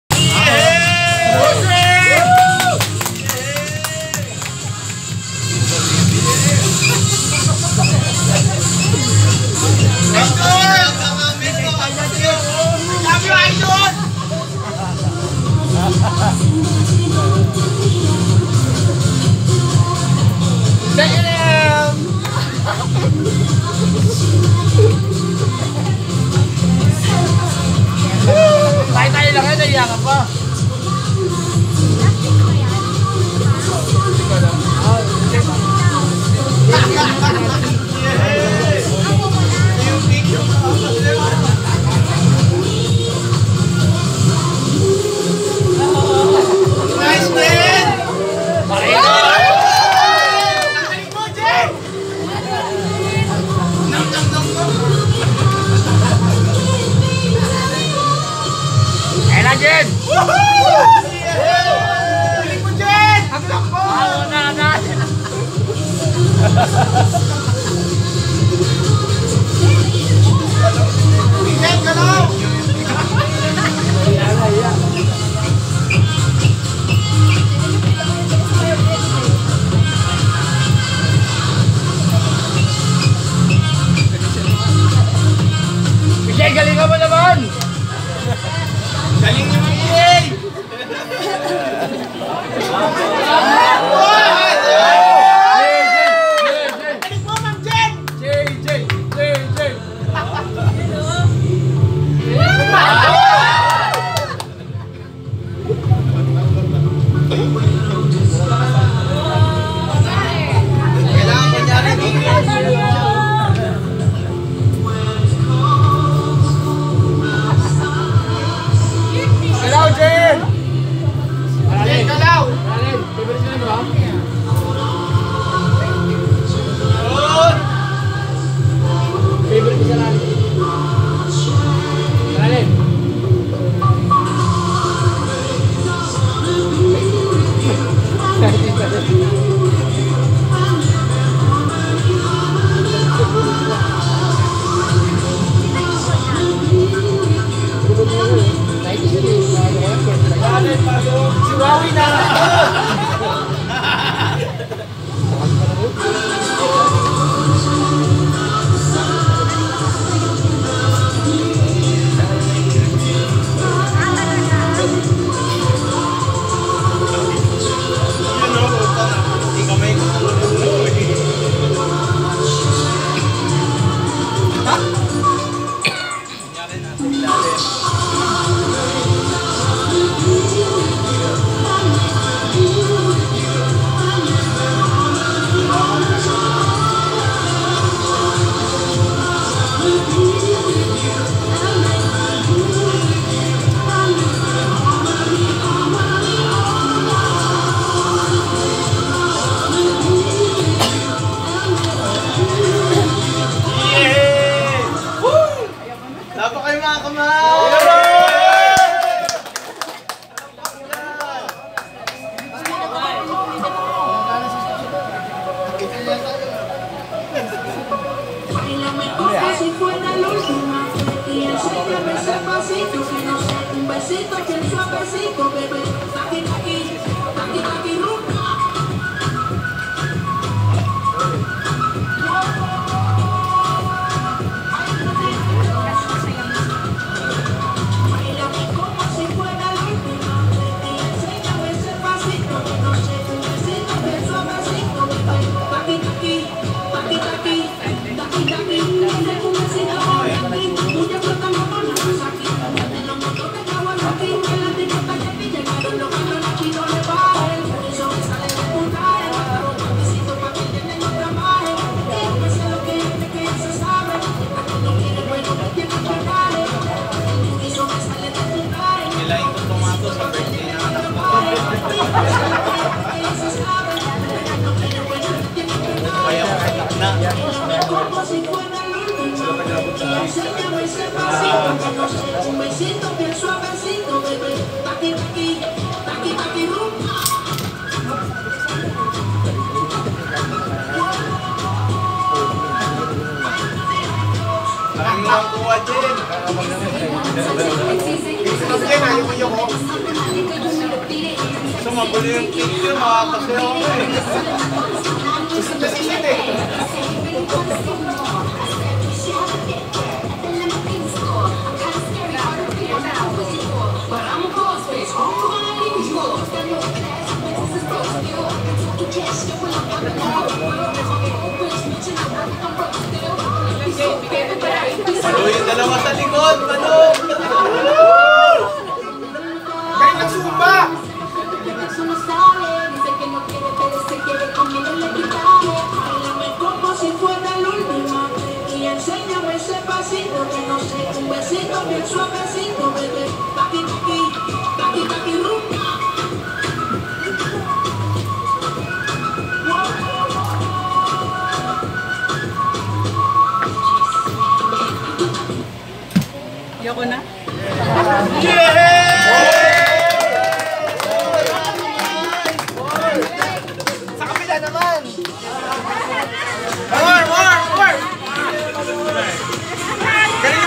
NSPI Dialysis Center West Avenue QC Christmas Party 2019
Intermission number or staffs and nurses last December 20, 2019